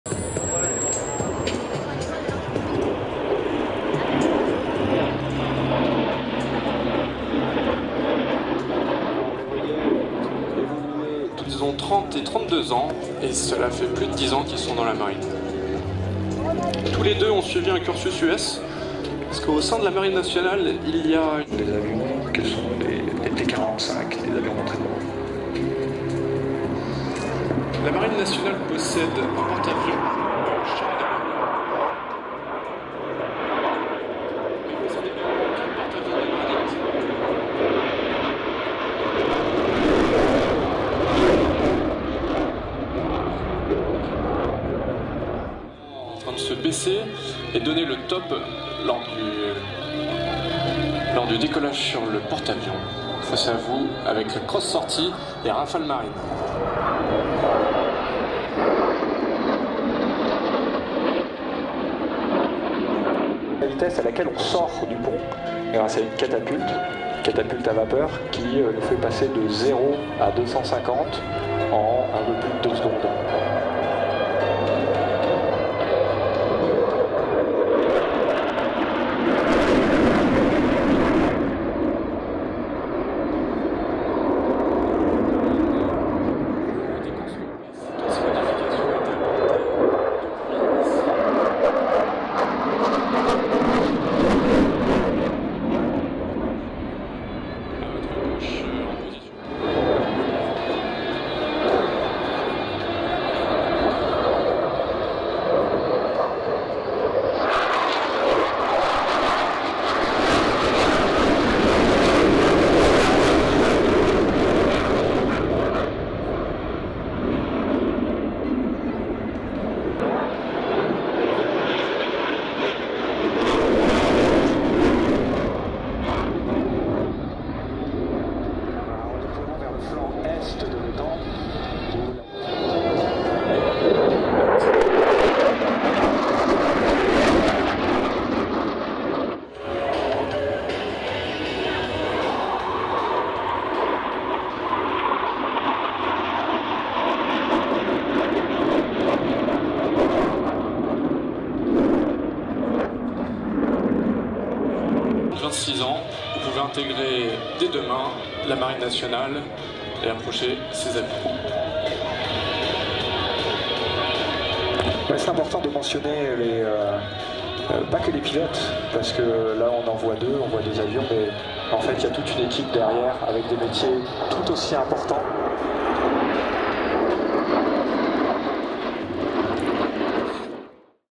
Rafale Marine Rassemblement International d'Hydravions Biscarrosse 28 Mai 2022
Le rassemblement international d'hydravions de Biscarrosse a lieu tous les deux ans. C'est le lieu idéal pour voir évoluer des Hydravions mais aussi des appareils en rapport avec la mer. C'est le cas des Rafale Marine qui sont venus faire une démo au dessus du lac de Biscarrosse. Bien qu'en contre jour nous voyons ici les avions évoluer devant nous. Place au Rafale M